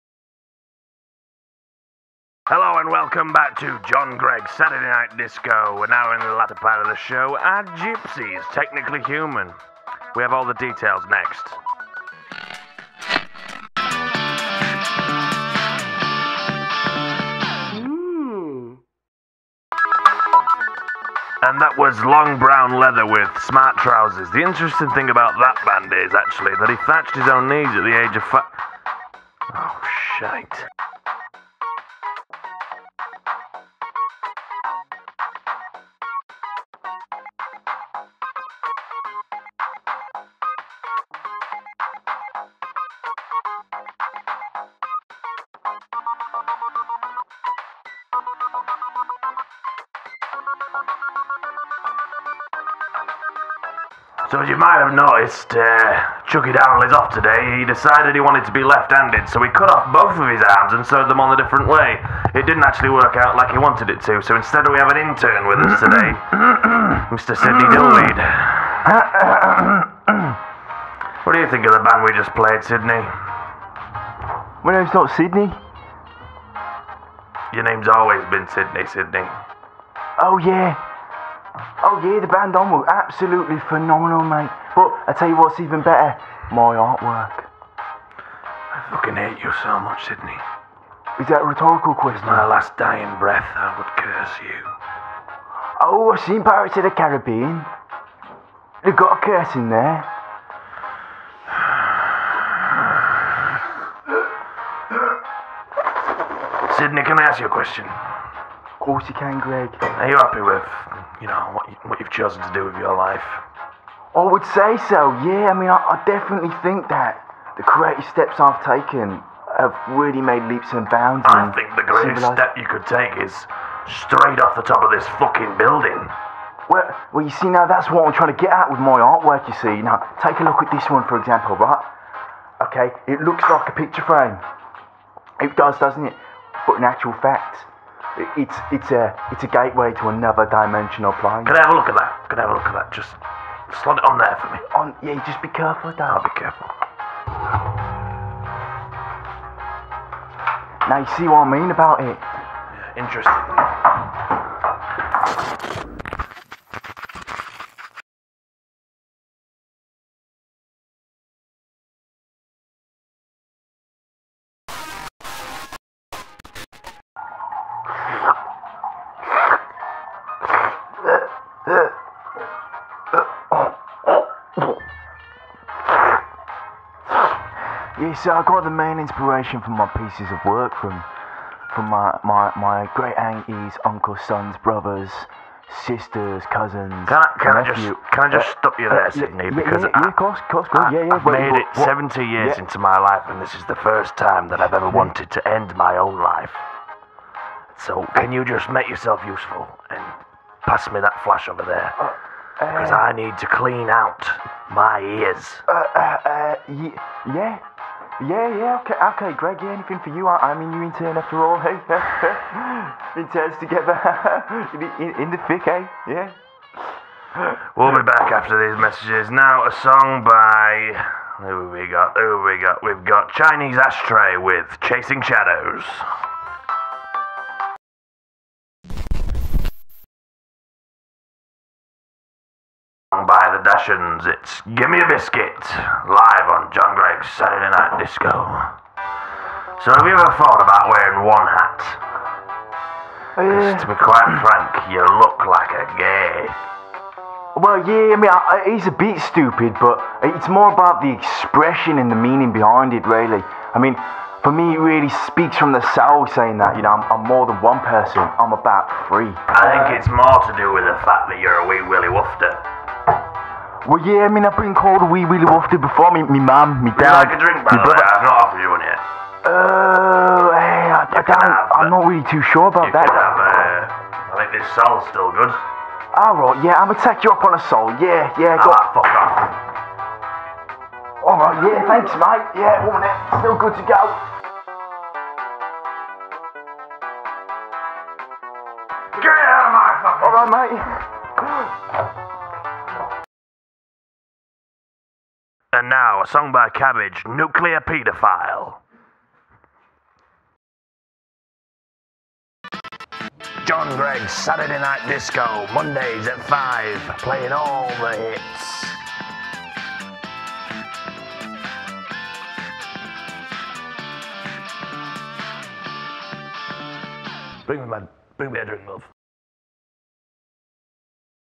John Gregg's Saturday Night Disco
John Gregg is the most famous man in all of Cleethorpes (and some parts of Grimsby), making his fortune from whelk stands on the pier in his youth, John then rose to stardom hosting local talent nights in the area, as well as performing raffles at the local community centre and even commentating for the Darts championships from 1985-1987. Together with his musician friend Chucky Darrell (Chinese Ashtray) and intern Sydney Dillweed they host “John Greggs Saturday Night Disco” at the world famous Cleethorpes One Radio Station, playing songs old and new, discussing the news and answering listener calls.

John Hopes that one day he will break free from the chains of local radio, and hit the big time, taking his talent shows on the road, and eventually on the Television!